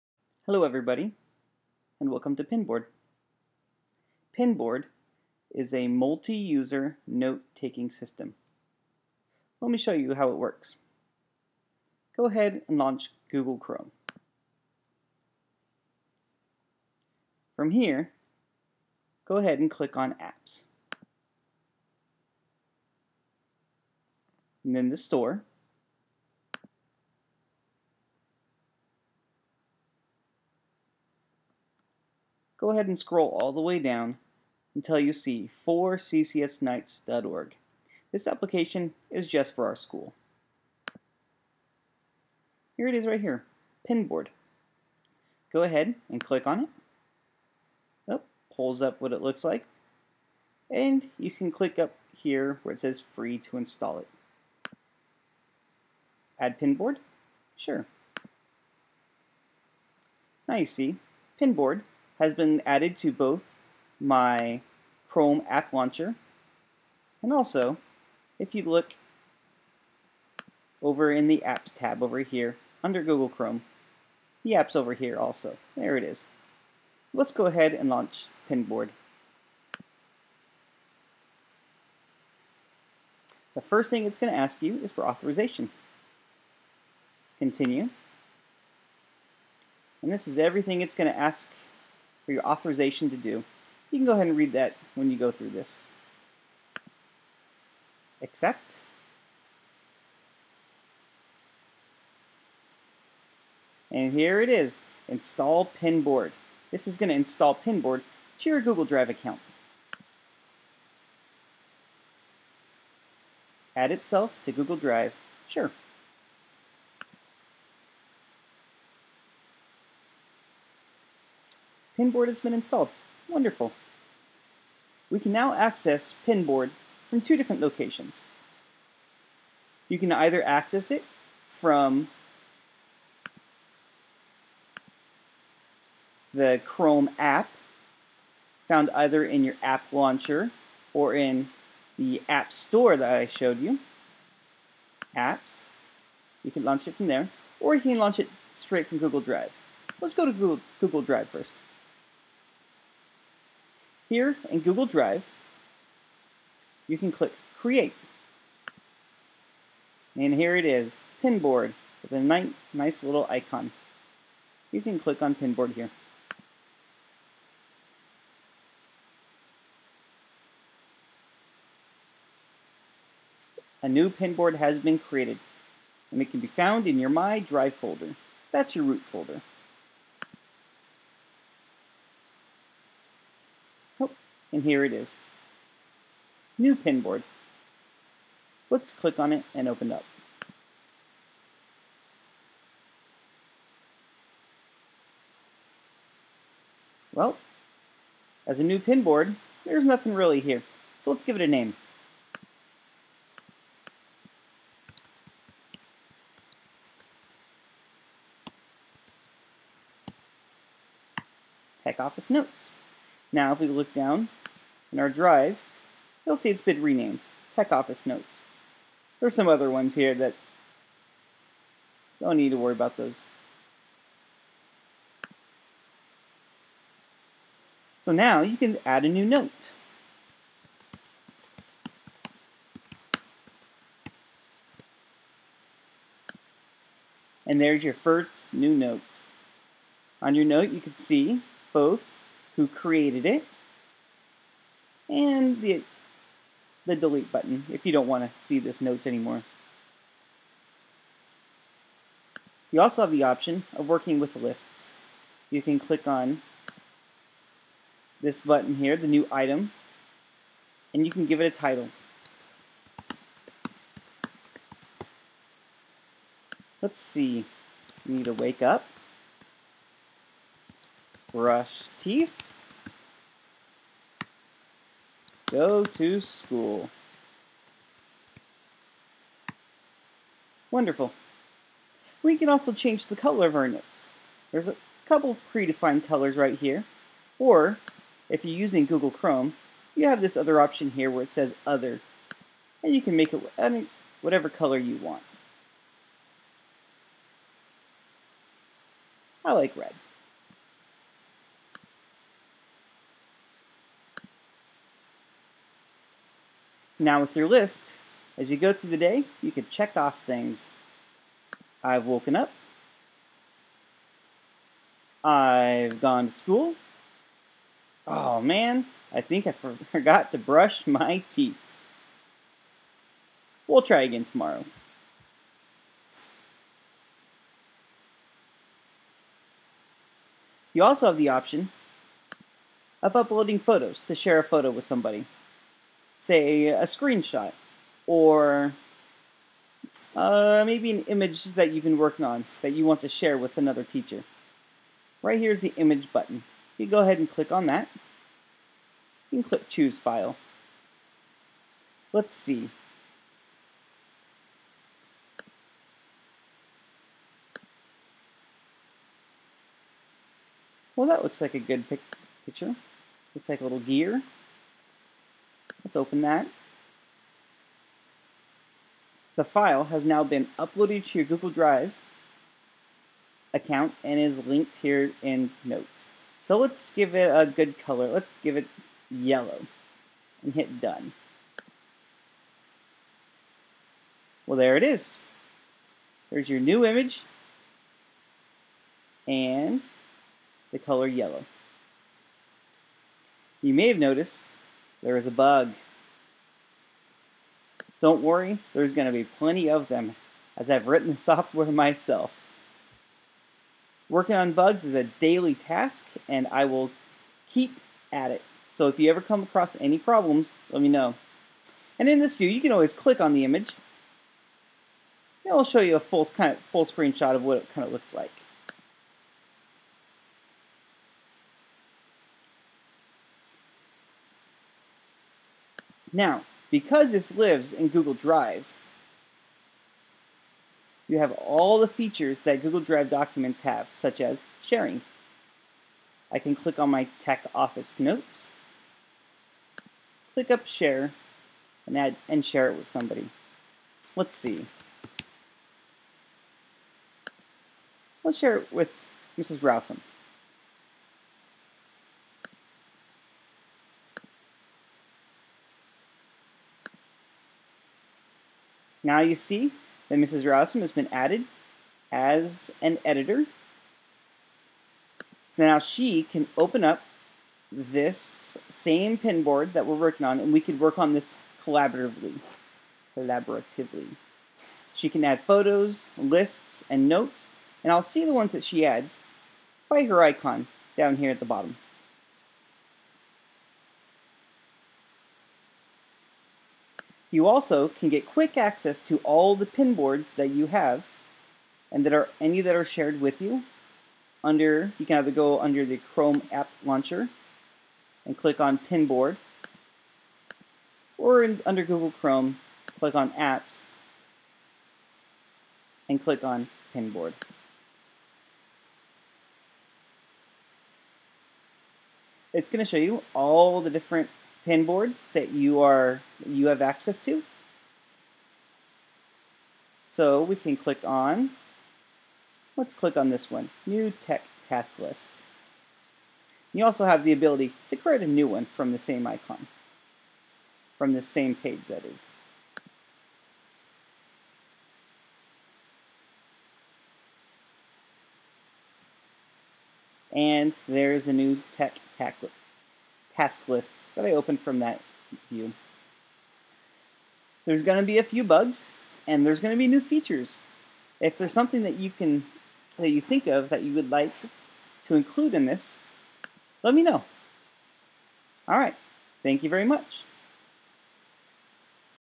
Pinboard Demonstration
Learn how to install Pinboard and how to use some of its core features